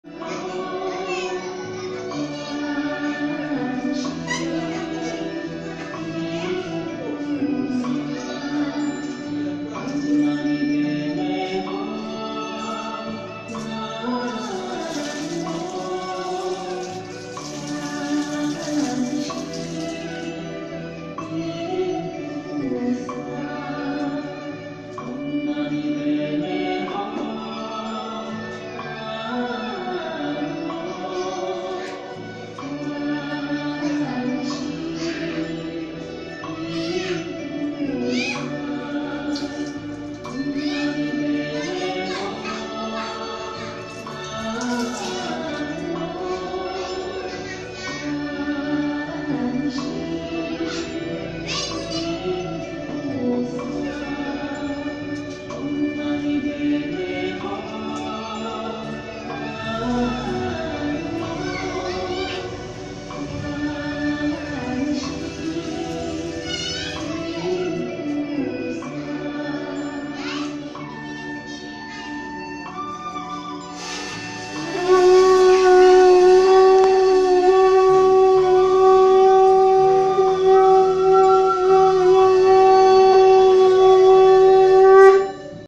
Memandikan Arca Patung Buddha
Menjelang Perayaan Tri Suci Waisak, dengan Sankhakala simbolis Air dari Samudra